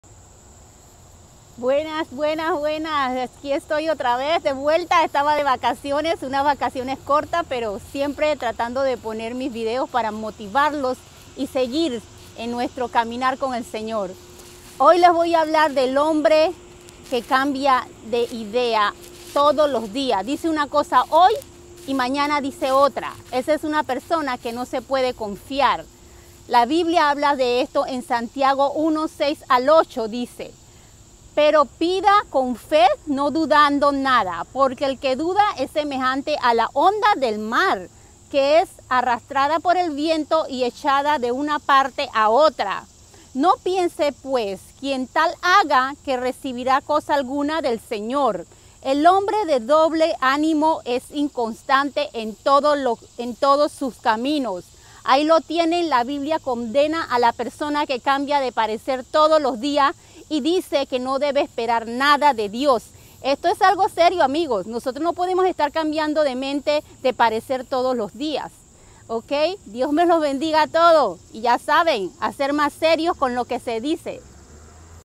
Santiago 1:6-8 “…El hombre de doble ánimo es inconstante en todos sus caminos.
Santiago 1:6-8 Reina-Valera

Pero pida con fe, no dudando nada; porque el que duda es semejante a la onda del mar, que es arrastrada por el viento y echada de una parte a otra. No piense, pues, quien tal haga, que recibirá cosa alguna del Señor. El hombre de doble ánimo es inconstante en todos sus caminos.